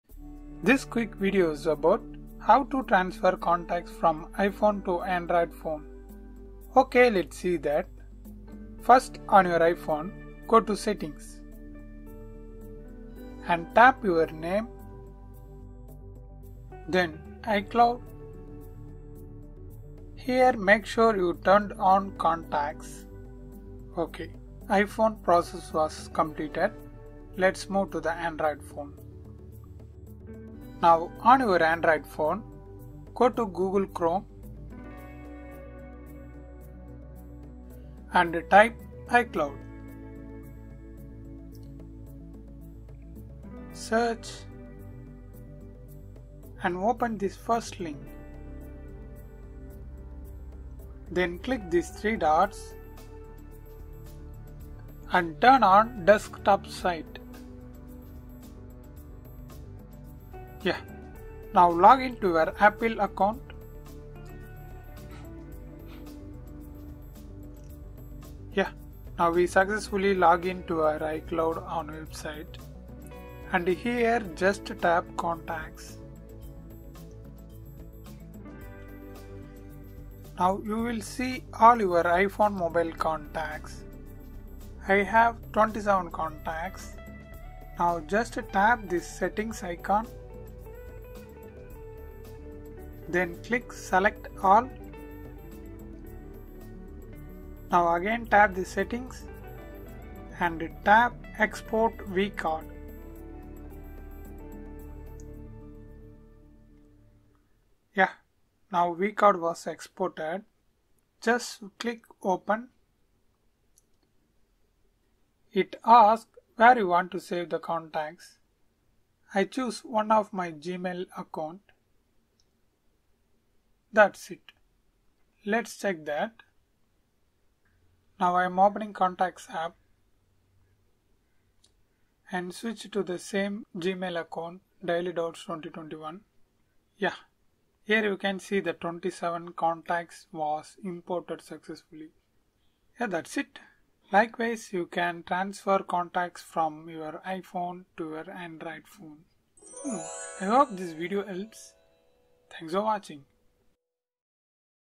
How to Transfer Contacts from iPhone to Android Phone?
How to Transfer Contacts from iPhone to Android Phone? In this video, I will show you how to transfer contacts from iphone to an android phone. Actually, You can easily transfer all your contacts from iphone to your android phone.